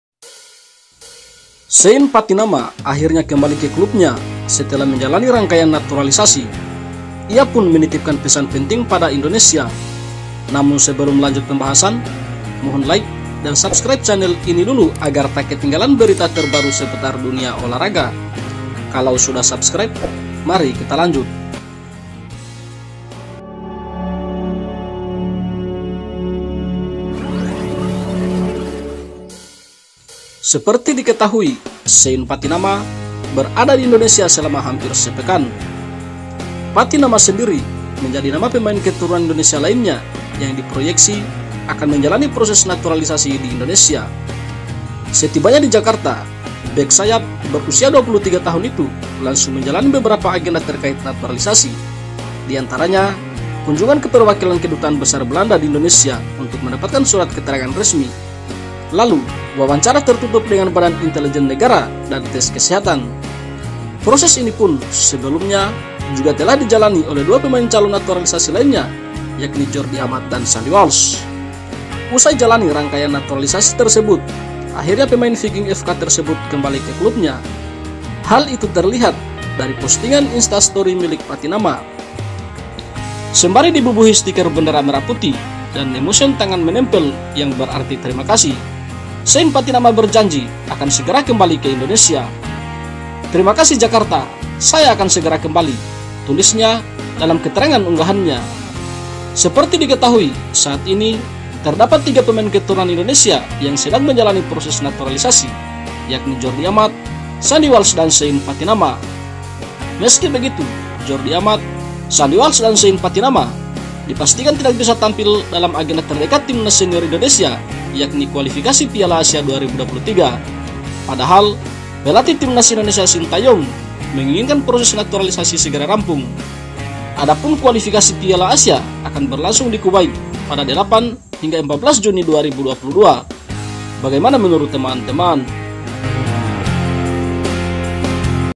🔴 Tinggalkan Indonesia dan Kembali Ke Klub, Shayne Pattynama Titip Pesan Penting Ini Pada Indonesia
Seperti diketahui, Shayne pattynama berada di Indonesia selama hampir sepekan.pattynama sendiri menjadi nama pemain keturunan Indonesia lainnya yang diproyeksi akan menjalani proses naturalisasi di Indonesia.
Setibanya di Jakarta, bek sayap berusia 23 tahun itu langsung sibuk menjalani beberapa agenda terkait naturalisasi. Di antaranya kunjungan ke perwakilan Kedutaan Besar Belanda di Indonesia untuk mendapatkan surat keterangan resmi.

Lalu, wawancara tertutup dengan Badan Intelijen Negara (BIN) dan tes kesehatan. Proses ini sebelumnya juga telah dijalani oleh dua pemain calon naturalisasi lainnya, Jordi Amat dan Sandy Walsh.
Usai jalani rangkaian naturalisasi tersebut akhirnya pemain viking FK terebut kembali ke klubnya..hal itu terlihat dari postingan insta story milik pattynama..
Sembari dibubuhi stiker bendera merah putih dan E-motion  dia tangan menempel yang berarti terima kasih...
"Terima kasih Jakarta, saya akan segera kembali" tulisnya dalam keterangan unggahannya.

Seperti diketahui saat ini terdapat tiga pemain keturunan Indonesia yang sedang menjalani proses naturalisasi yakni Jordi Amat, Sandy Walsh dan terakhir Shayne Pattynama.
Meski begitu, Jordi Amat, Sandy Walsh, dan Shayne  tidak bisa tampil dalam agenda terdekat Timnas Senior Indonesia, yakni di Kualifikasi Piala Asia 2023.

Padahal, pelatih Timnas Indonesia, Shin Tae-yong menginginkan proses naturalisasi segera rampung. Adapun Kualifikasi Piala Asia akan berlangsung di Kuwait pada 8-15 Juni 2022.